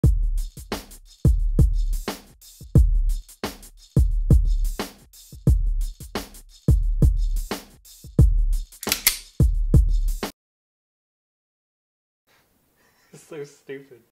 Kevin Has a New Weapon
Kevin has armed himself after the run it at WalMart. May God help us all.